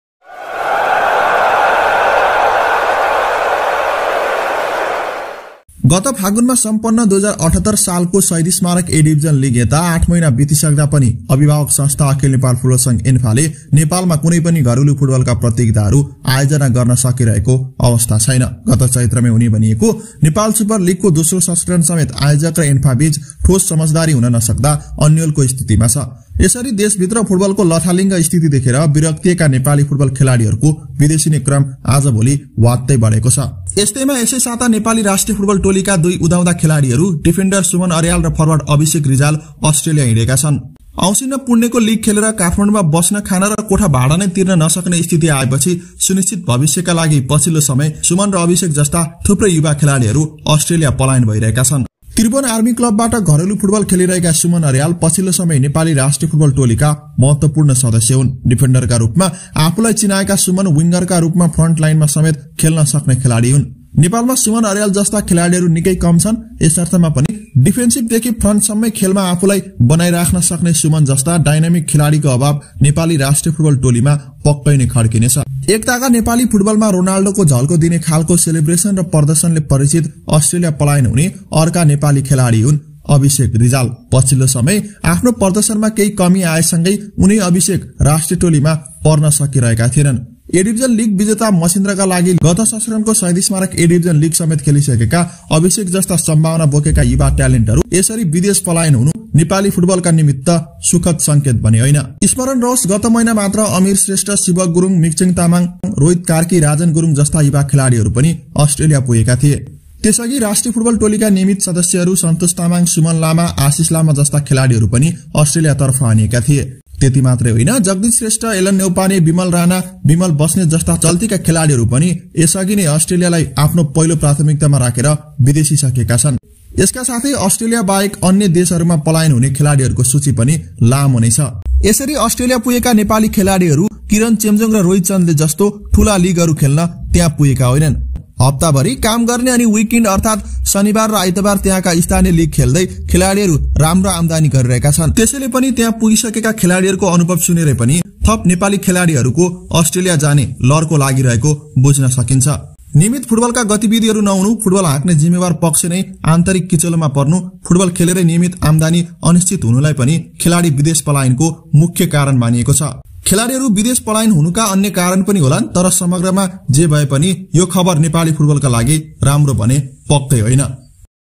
Australia उडे Suman Aryal र Abhisek Rijal | धमाधम बाहिरिदै Nepali खेलाडीहरु | के हेर्दै छ ANFA?
Two more Nepal internationals defender Suman Aryal & forward Abhisek Rijal have left Nepal for Australia.

Aryal is regular in the national team whereas Rijal is on-off in the team.

Rajan Gurung, Rumesh Bartaula, Rabi Thing, Amir Shrestha and Mikchhen Tamang have recently flown to Australia. Similarly, Santosh Tamang, Darshan Gurung, Bimal Basnet, Jagjeet Shrestha, Heman Gurung, Bimal Rana, Rohit Karki and Alan Neupane are also in Australia.

Nepal's weak football structure and difficulties in making a living out of it are making the players leave Nepal. Only time will tell where Nepali football is heading.

I don't owe the photos and videos used in the video. Here are the photos & video credit: Armika Rijal, Abhisek Rijal, ANFA, Goalnepal, Annapurna Post, Kathmandu Post